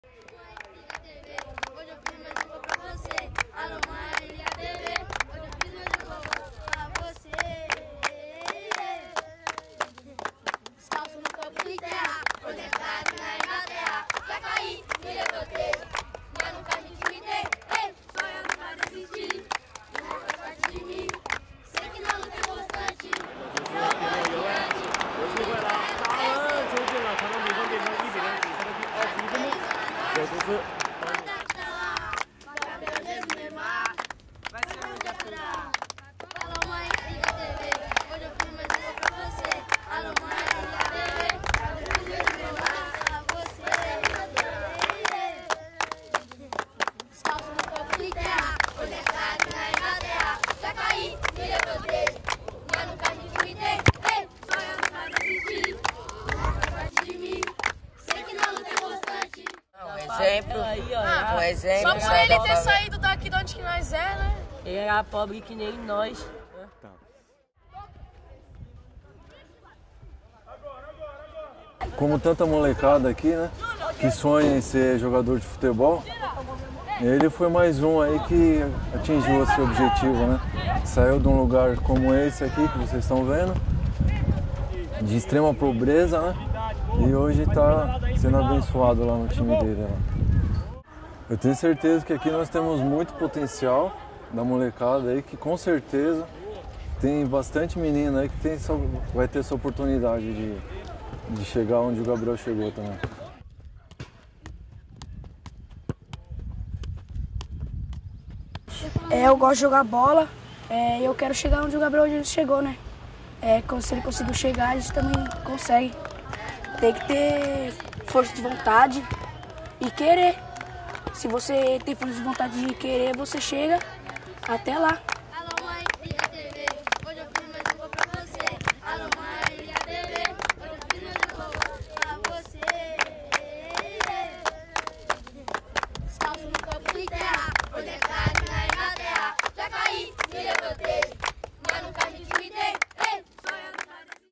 Brazil favela honors native son Gabriel Jesus with mural
As the globe tunes in to the World Cup, the people of Brazil's Jardim Peri favela outside of Sao Paulo have their eyes set on native son Gabriel Jesus, a striker on the Brazil team and Manchester United. CGTN's Paulo Cabral talks to kids in the favela about how their learning from Jesus's life story.

Follow CGTN America:
Google+: CctvamericaTvnews

»» Watch CGTN ««